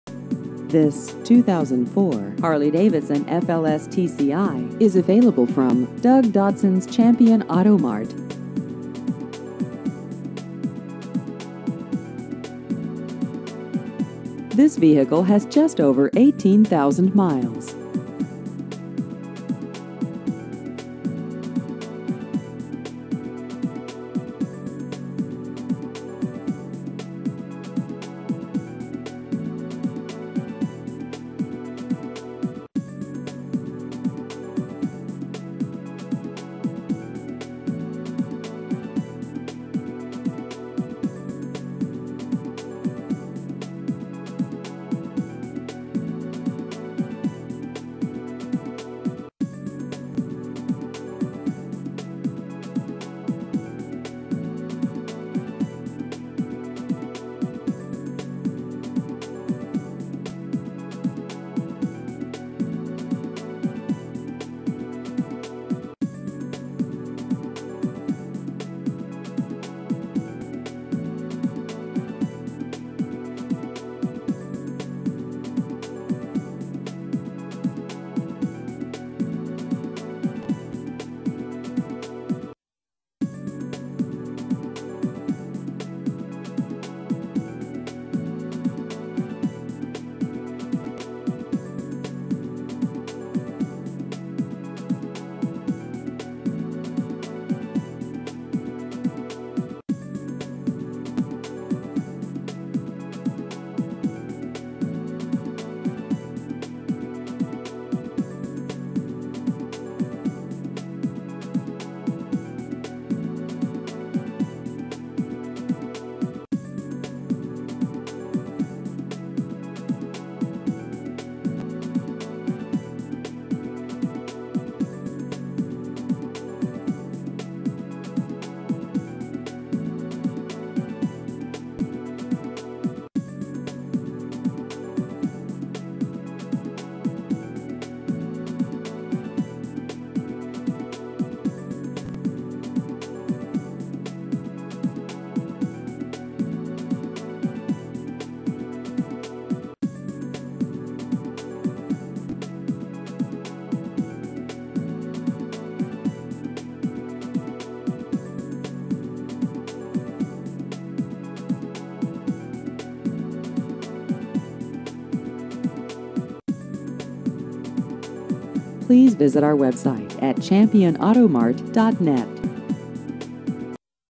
2004 Harley-Davidson FLSTCI Used Cars Benton KY
This 2004 Harley-Davidson FLSTCI is available from Doug Dotson's Champion Auto Mart.